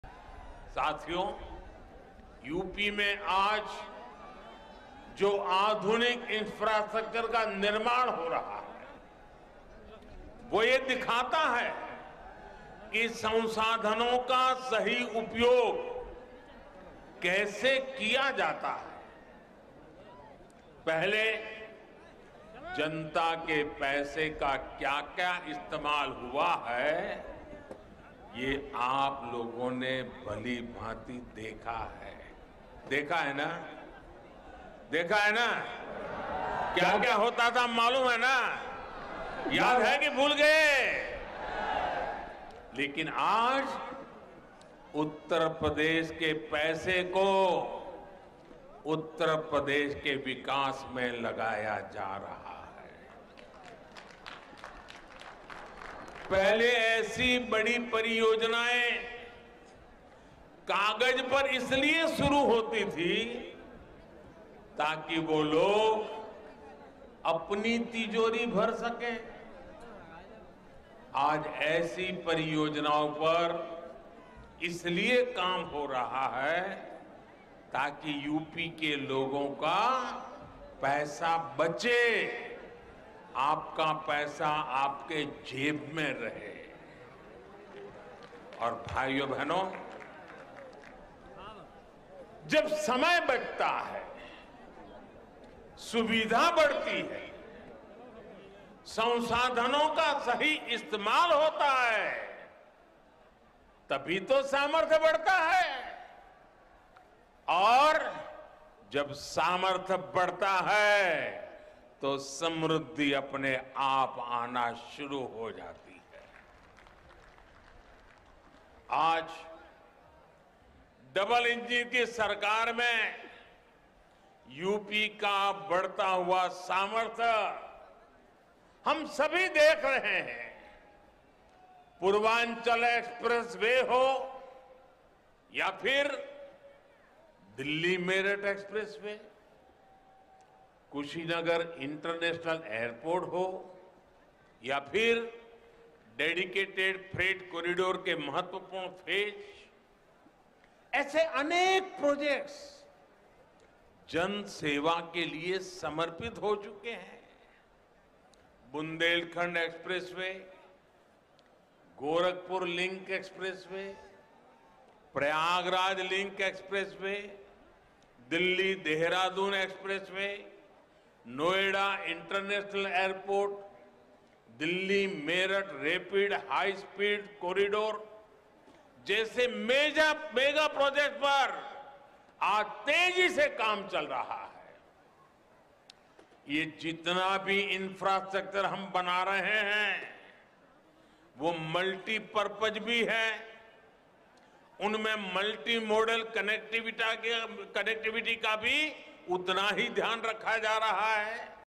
Modern infrastructure in Uttar Pradesh proves resources are being used effectively: PM
Prime Minister Shri Narendra Modi laid the foundation stone of Ganga Expressway in Shahjahanpur, Uttar Pradesh.

Addressing the gathering, the Prime Minister started with paying tribute to revolutionaries of the Kakori incident Ram Prasad Bismil, Ashfaq Ullah Khan and Roshan Singh. Speaking in the local dialect, the Prime Minister paid homage to Damodar Swaroop ‘Vidrohi’, Raj Bahadur Vikal and Agnivesh Shukla, poets of freedom struggle. “Tomorrow is the Martyrdom  Day of Pandit Ram Prasad Bismil, Ashfaq Ullah Khan, Thakur Roshan Singh. These three sons of Shahjahanpur, who challenged British rule, were hanged on 19 December. We owe a huge debt to such heroes who laid down their lives for the freedom of India”, The Prime Minister said.

The Prime Minister remarked that Maa Ganga is the source of all auspiciousness and all progress. Maa Ganga gives all the happiness, and takes away all the pain. Similarly, Ganga Expressway will be a source of five boons for the state, said the Prime Minister, referring to the network of expressways, new airports and railway routes. First boon - saving people's time. Second boon- Increase in convenience and ease of people. Third boon- Proper use of resources of UP. Fourth boon- Increase in the capabilities of UP. Fifth boon - all-round prosperity in UP.

The Prime Minister remarked that the modern infrastructure that is being built in UP today shows how the resources are used properly. “You have clearly seen how public money was used earlier. But today the money of Uttar Pradesh is being invested in the development of Uttar Pradesh”, the Prime Minister emphasized. The Prime Minister said when the whole of UP grows together, the country progresses. Therefore, the focus of the double engine government is on the development of UP.  ‘With the mantra of Sabka Saath, Sabka Vikas, Sabka Vishwas and Sabka Prayas, we are making sincere efforts for the development of UP”, he said. The Prime Minister brought the focus to the situation five years ago. “Except in some areas of the state, electricity was not available in other cities and villages. The government of double engine has not only given about 80 lakh free electricity connections in UP, but every district is being given many times more electricity than before”, he added. He said more than 30 lakh poor people got pucca houses and this campaign will continue to cover all the remaining entitled beneficiaries. In Shahjahanpur also 50 thousand pucca houses were built.

For the first time development of dalits, deprived and backward is being prioritized at his level, he said. He added “the priority of the government is to bring the benefits of development  to whoever is left behind in the society and  is backward.The same sentiment is reflected in our agriculture policy and  in the policy related to farmers”.

He recalled the abysmal law and order situation in the state that changed for the better in recent times. The Prime Minister gave the formulation U.P.Y.O.G.I - UP plus Yogi bahut hai upyogi (is very useful).

The inspiration behind the Expressway is the vision of the Prime Minister to provide fast paced connectivity across the country. The 594 Km long six-lane Expressway will be built at a cost of over Rs 36,200 crore. Starting near the Bijauli village in Meerut, the Expressway will extend till near the Judapur Dandu village in Prayagraj. It passes through Meerut, Hapur, Bulandshahr, Amroha, Sambhal, Budaun, Shahjahanpur, Hardoi, Unnao, RaeBareli, Pratapgarh and Prayagraj. Upon completion of work, it will become the longest expressway of Uttar Pradesh, connecting the western and eastern regions of the state. A 3.5 Km long air strip for assisting emergency take off and landing of Air Force planes will also be constructed on the Expressway in Shahjahanpur. An industrial corridor is also proposed to be built along the Expressway.

The Expressway will also give a fillip to multiple sectors including industrial development, trade, agriculture, tourism etc. It will provide a big boost to the socio-economic development of the region.

pm modi in shahjahanpur
ganga expressway shahjahanpur
pm modi visit shahjahanpur
latest uttar pradesh news